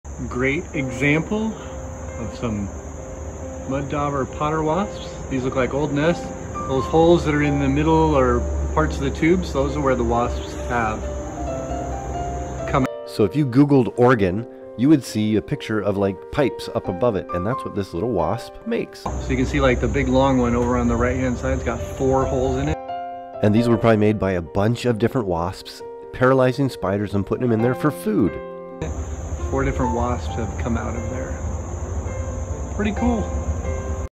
WASP NESTS Full of PREY! 🐝
These wasp nests are cool right! Many different Organ Pipe wasps built them and all side by side. The wasp nests have compartments in them where the wasp stores spiders that are STUNG bug still alive! Insects, nature, wasps and hymenoptera, all cool!